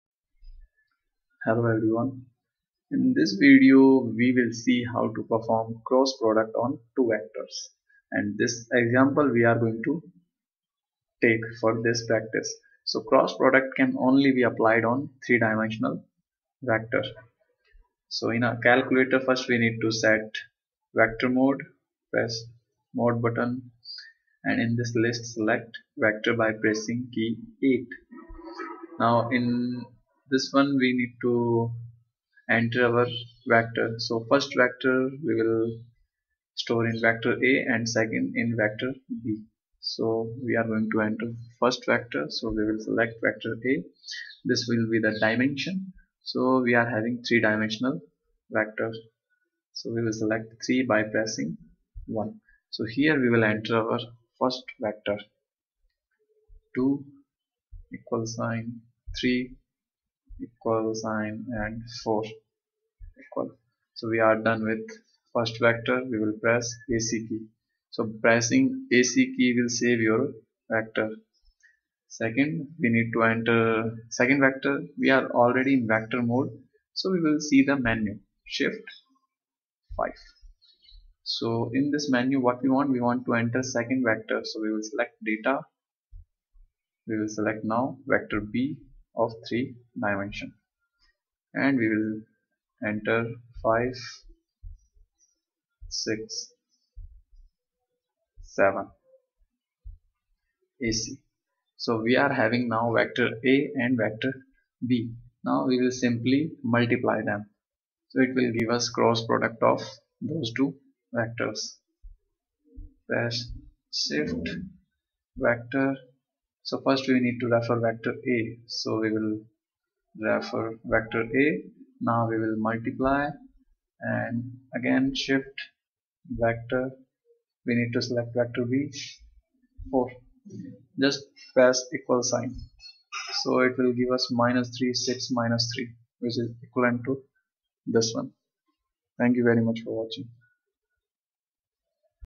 How to perform Cross Vector Product in Casio 991ES Calculator
In mathematics and vector calculus, the cross product or vector product (occasionally directed area product to emphasize the geometric significance) is a binary operation on two vectors in three-dimensional space (R3) and is denoted by the symbol ×. Given two linearly independent vectors a and b, the cross product, a × b, is a vector that is perpendicular to both a and b and therefore normal to the plane containing them. It has many applications in mathematics, physics, engineering, and computer programming. It should not be confused with dot product (projection product).